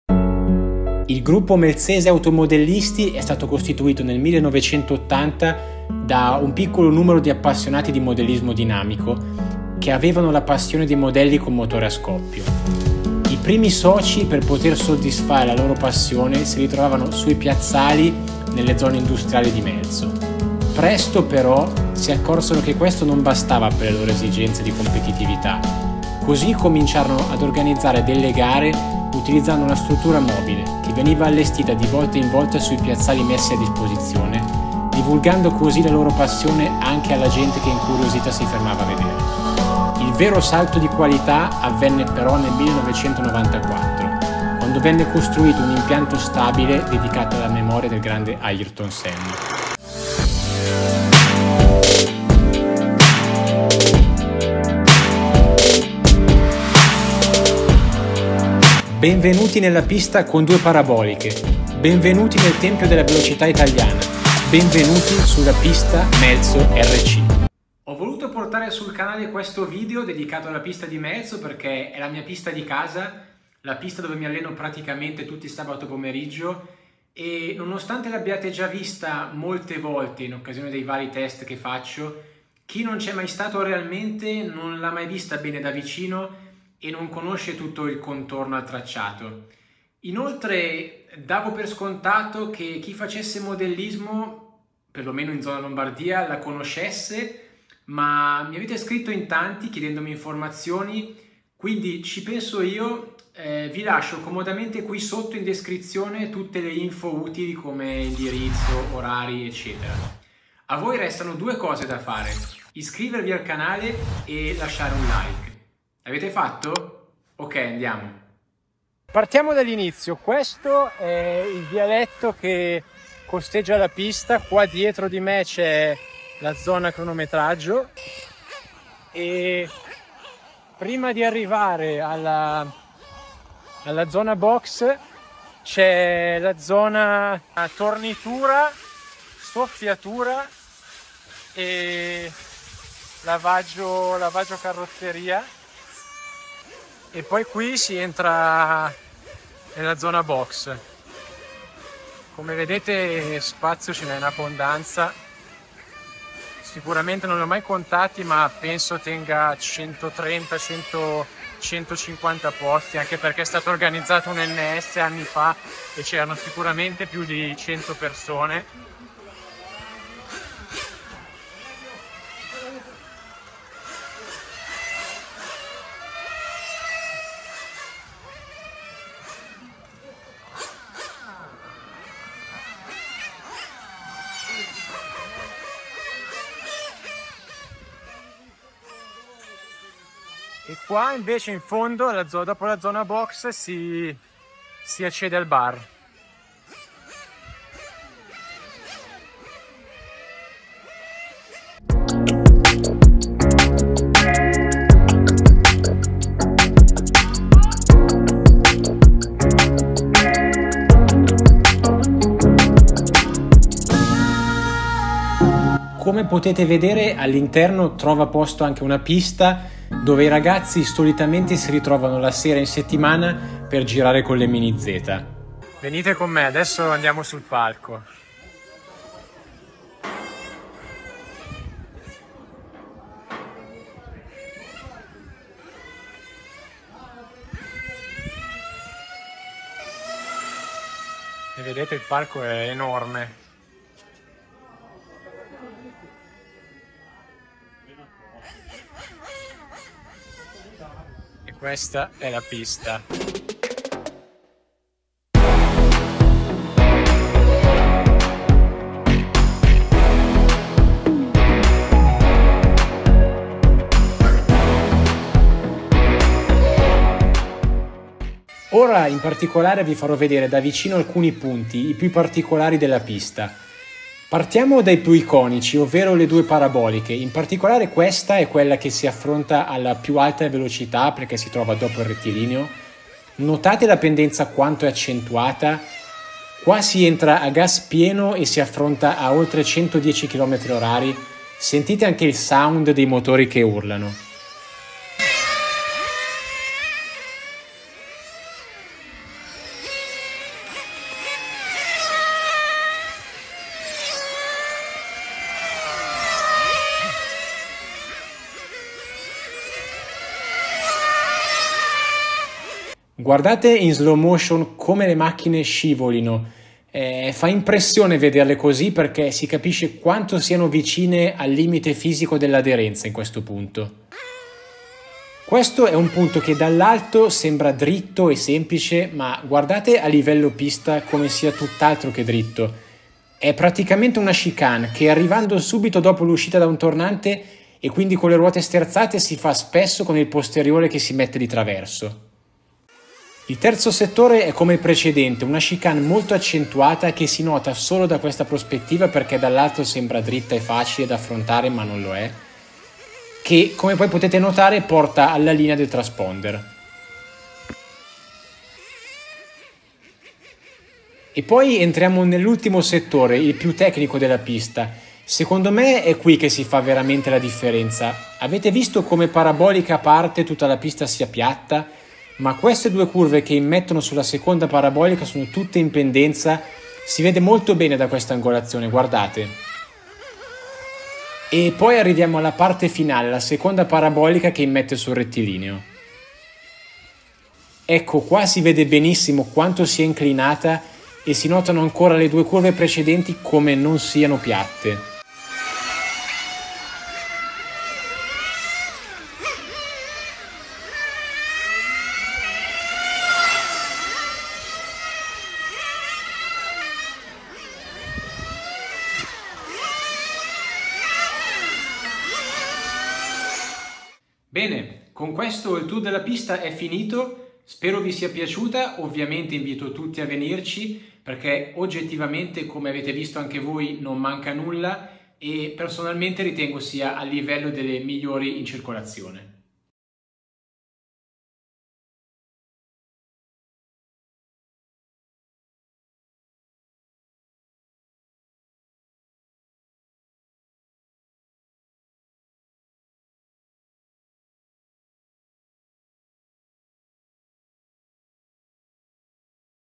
Pista MELZORC
Vi presento la mia pista di casa, con una panoramica di tutto l'impianto.
Tutte le info relative (orari di apertura, contatti dei gestori etc) le trovate sul sito ufficiale